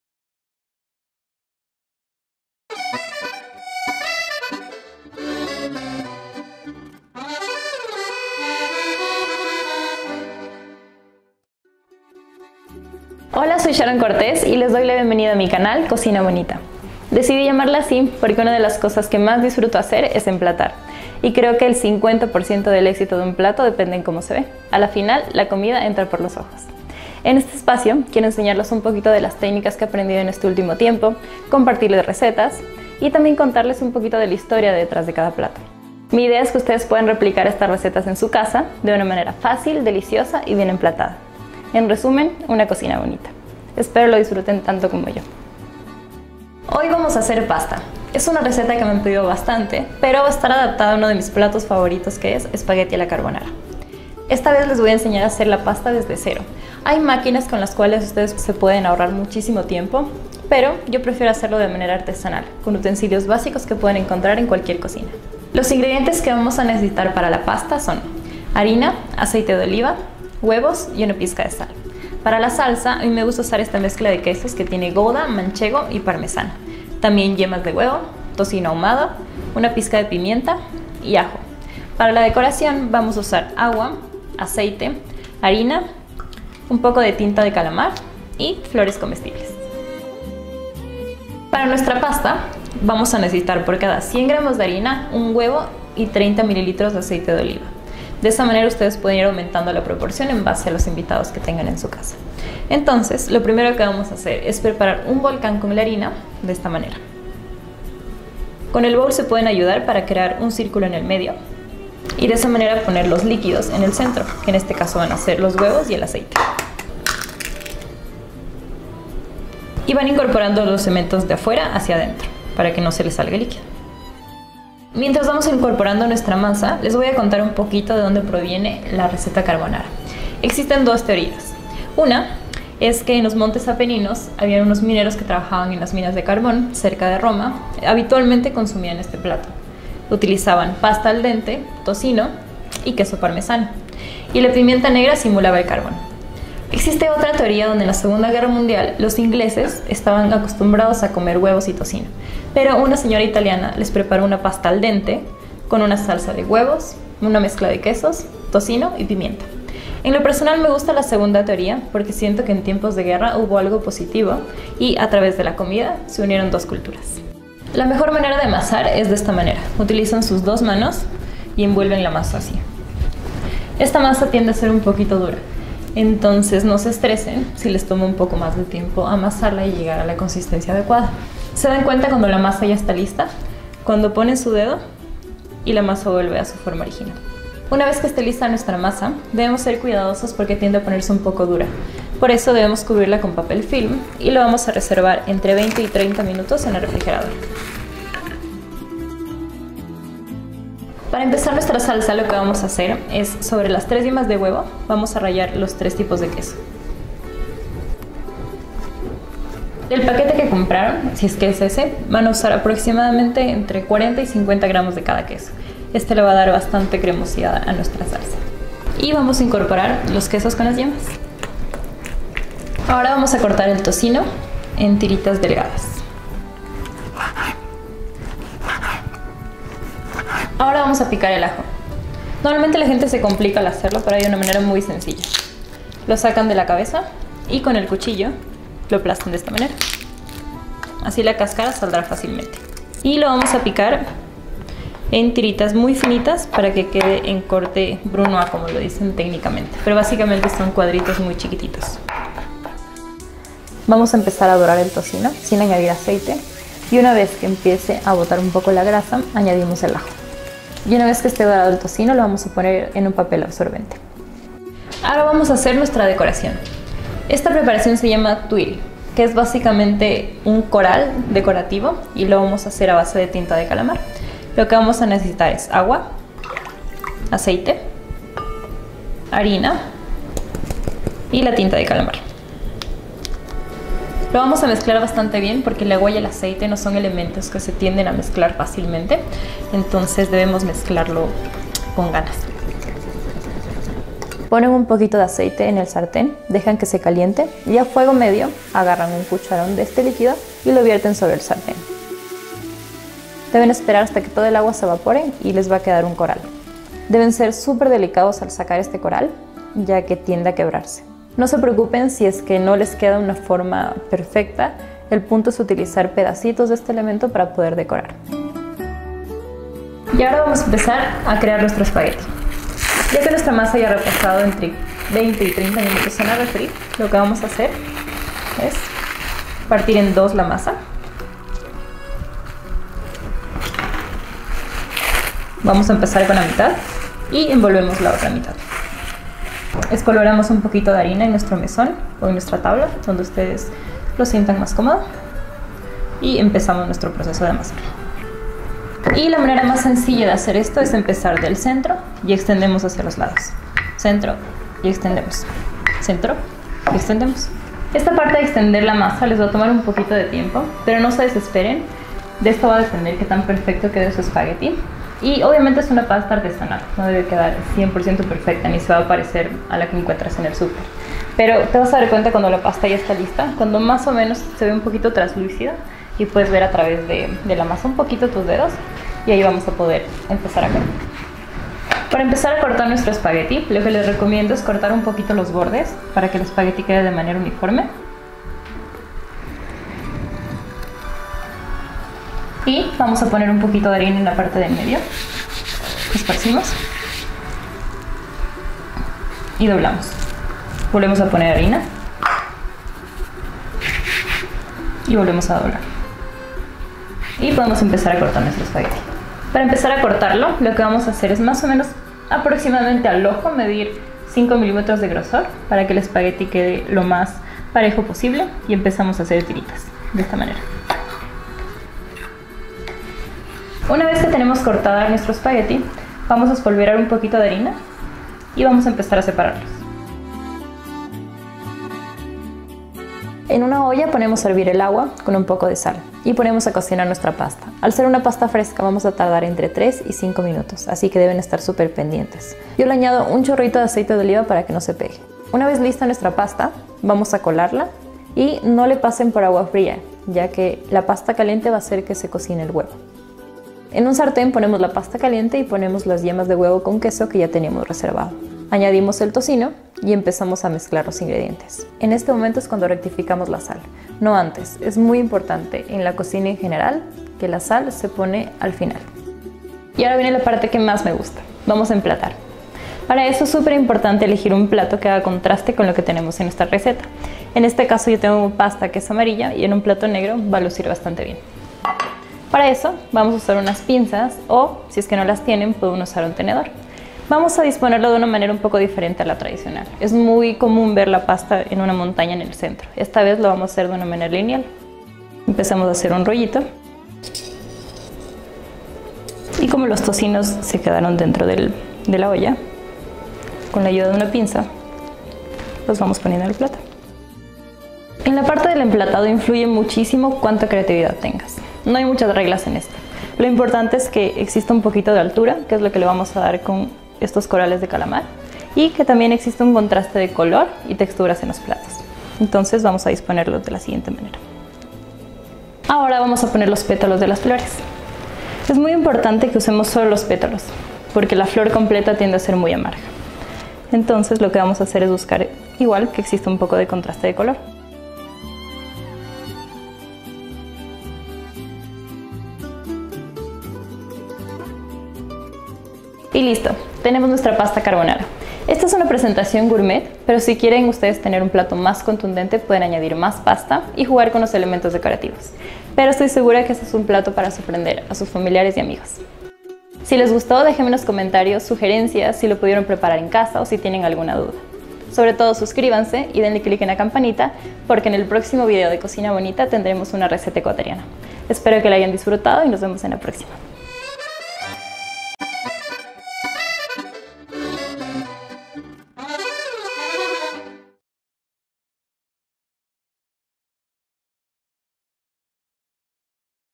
Pasta Carbonara con Sharon Cortez/ COSAS de Chef
La participante de Masterchef, Sharon Cortez, nos enseña paso a paso cómo hacer una de sus recetas favoritas. Pasta a la Carbonara